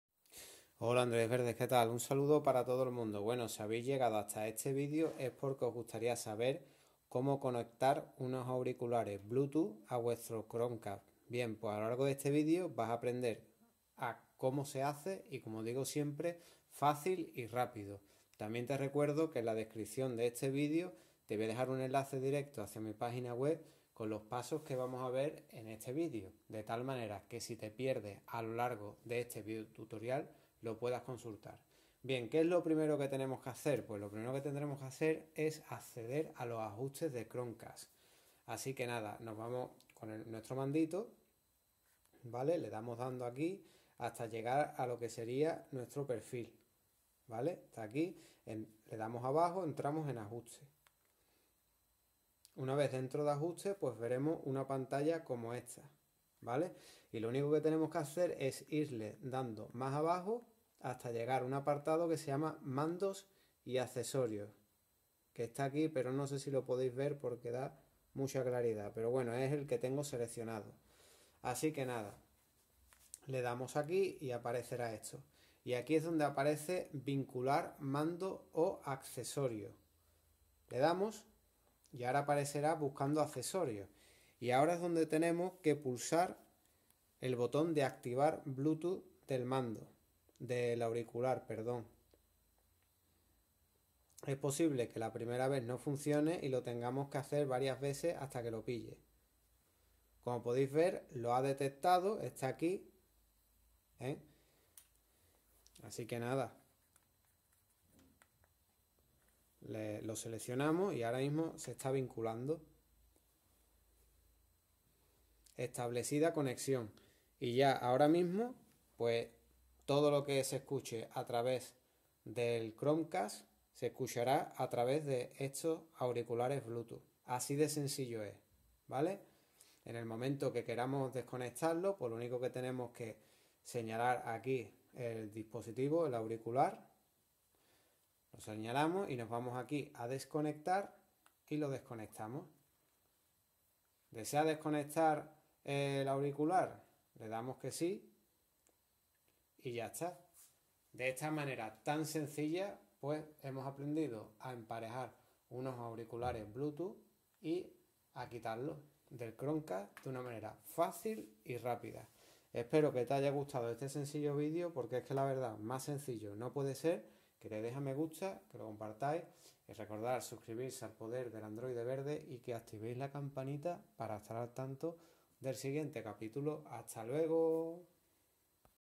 🎧 CONECTAR ALTAVOZ o AURICULARES BLUETOOTH a CHROMECAST con GOOGLE TV FÁCIL y RÁPIDO
En el siguiente  vídeo educativo te mostraremos como poder emparejar unos auriculares Bluetooth a Chromecast paso a paso.

Vídeos Relacionados 📹

Tutoriales ⚙️

Multimedia 📹